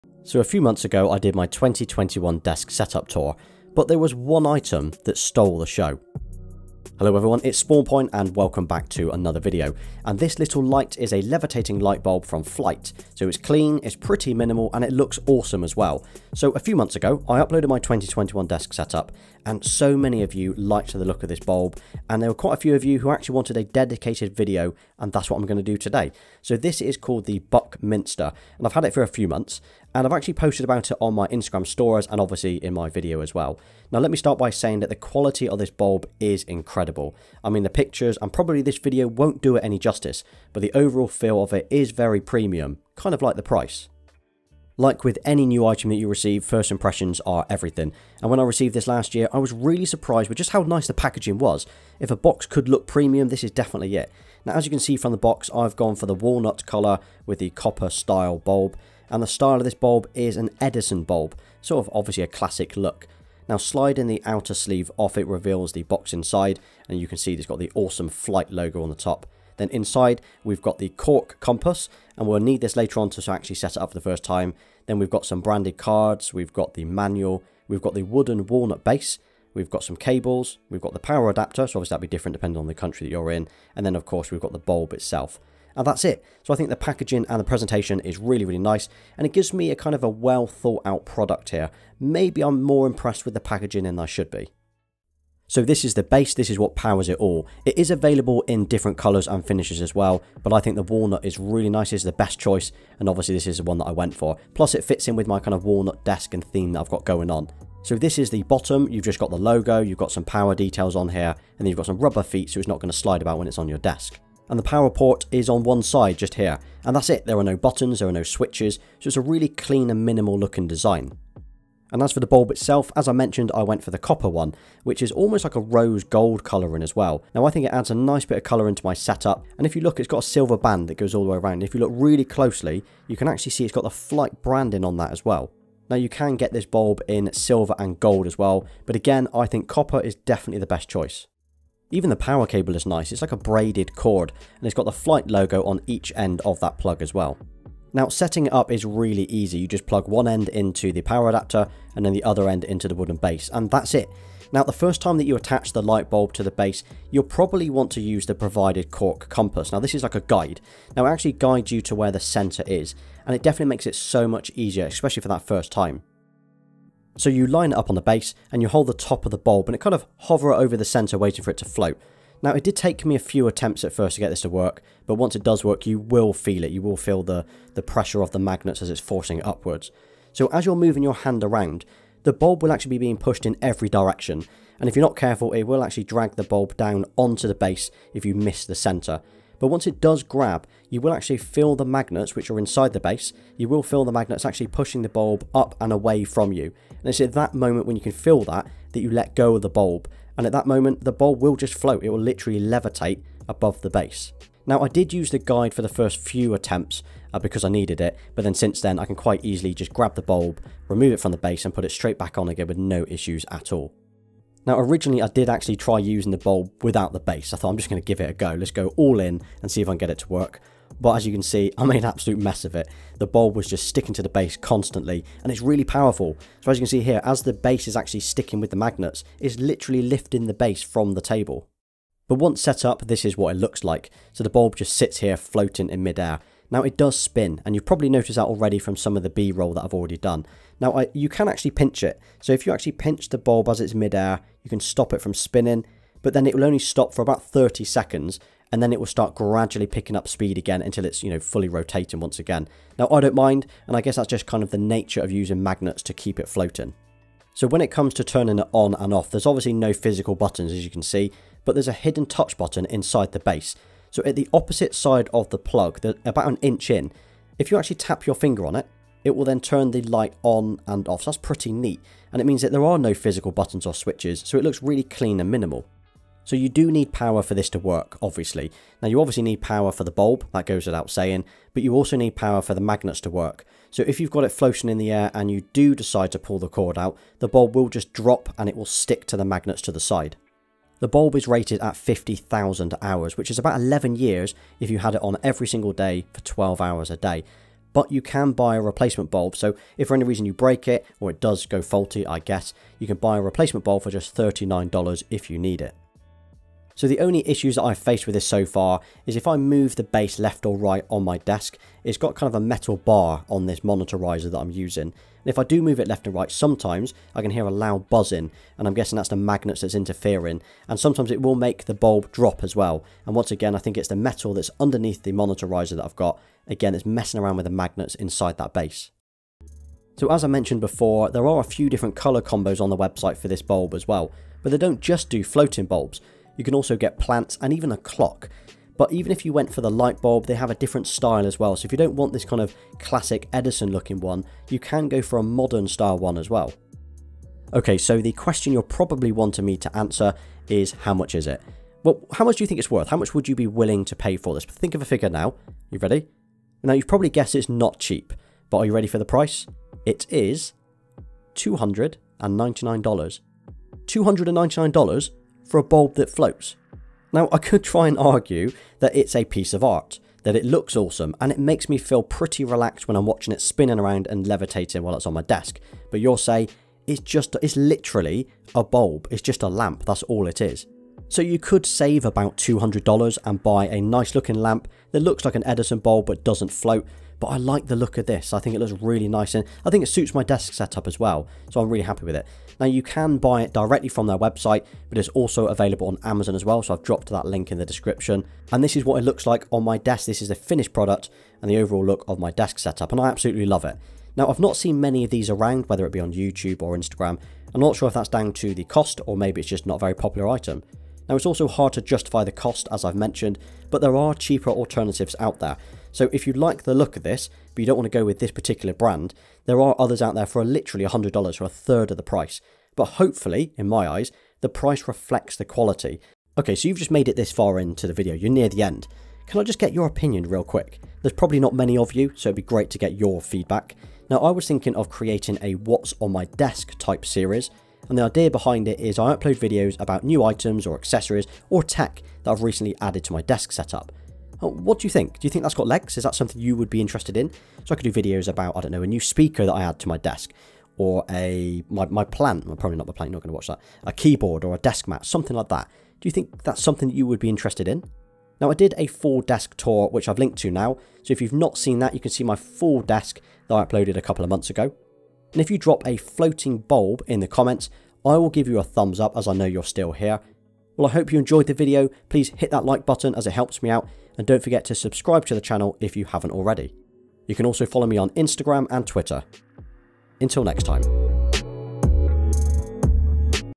The Levitating Light Bulb | On My Desk
The levitating light bulb got a lot of attention on my 2021 desk setup tour, so I thought it was time I showed it some light… this is it, the floating lamp from Flyte.

I’m thinking of creating a new series, a “What’s on my Desk” type series, where I upload videos of items and accessories on my desk, or new tech I buy for my desk setup. Let me know what you think.

I went for the copper and walnut LED Eddison version, which I think looks great. So here’s my quick unboxing, review, setup and impressions of this after a few months of use. It’s a magnetic bulb with touch buttons and is mains operated.